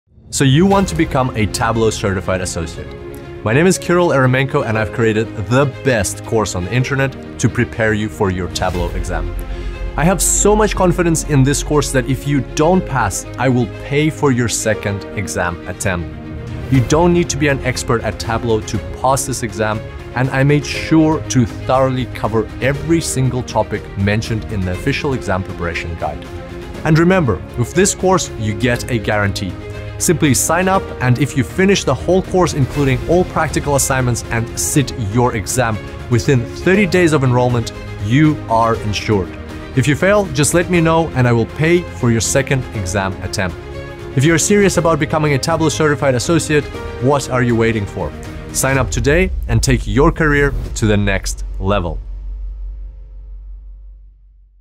Become Tableau Certified Associate: 50% Off Until June 30th
SuperDataScience’s best-selling Tableau Certified Associate Exam Guide is relaunching June 30, but it’s yours now for 50% off.

Enroll in our flagship course today and save $100 the list price before it moves to its new home: Art of Visualization.com — the ultimate training destination in data storytelling, where Tableau certification is guaranteed.

Be quick, this offer expires June 30!